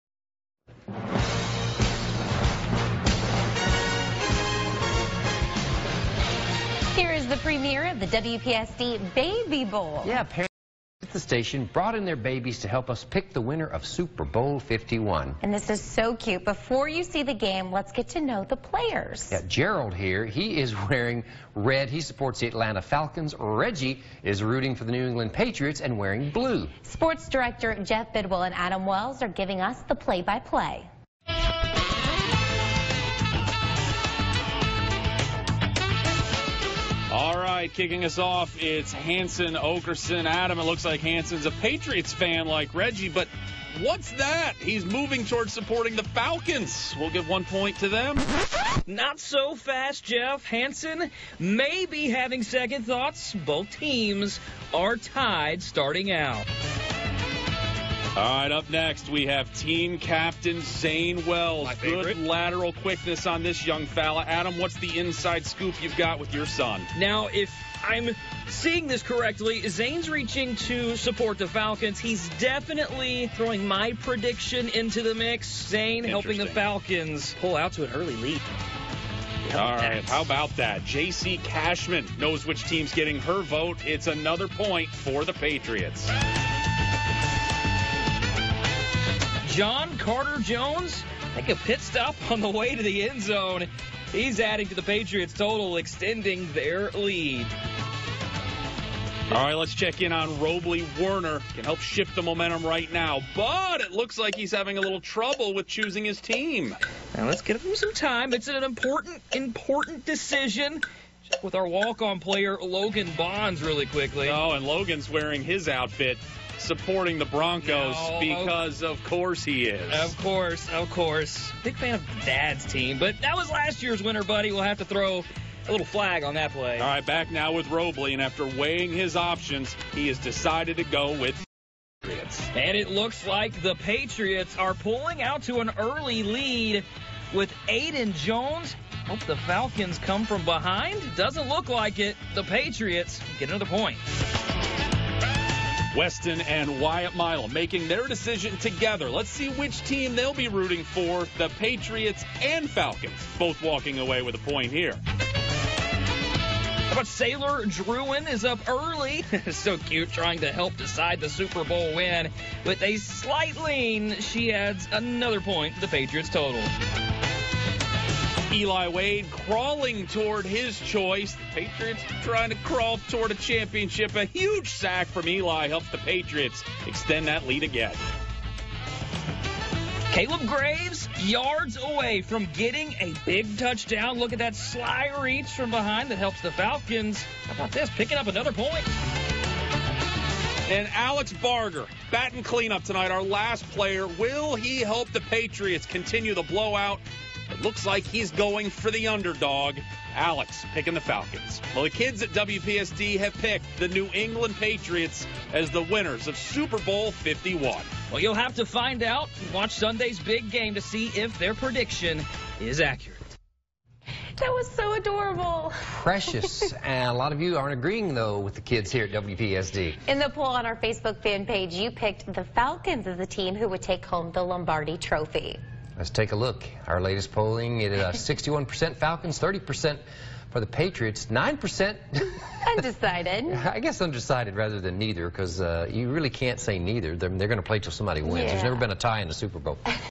WPSD Local 6 Baby Bowl 2017
The Kids At WPSD Predict the Super Bowl Winner